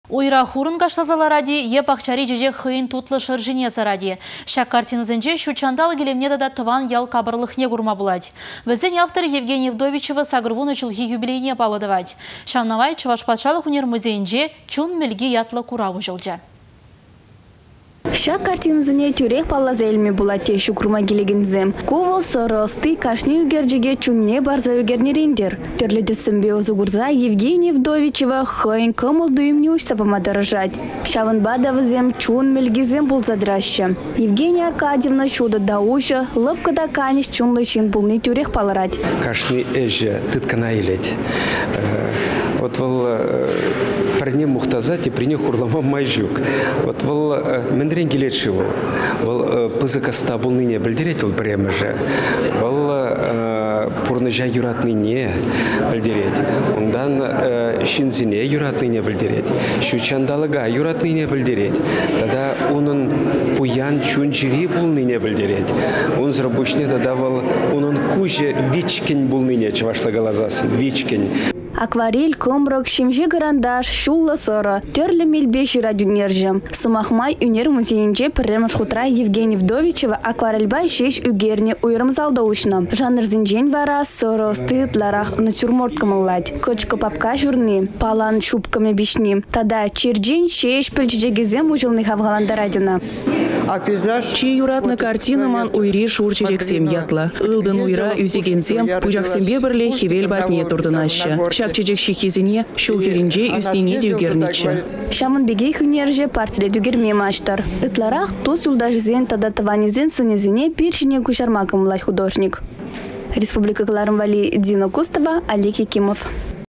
Евгения Вдовичева хăйĕн сумлă юбилейне паллă турĕ
Уйра хурăн кашласа ларать-и е пахчари чечек хăйĕн тутлă шăршине сарать-и. Çак картинăсенче çут çанталăк илемне тата тăван ял капăрлăхне курма пулать. Вĕсен авторĕ Евгения Вдовичева 80 çулхи юбилейне паллă тăвать. Çавна май Чăваш патшалăх ÿнер музейĕнче «Чун мĕлки» ятлă курав уçăлчĕ.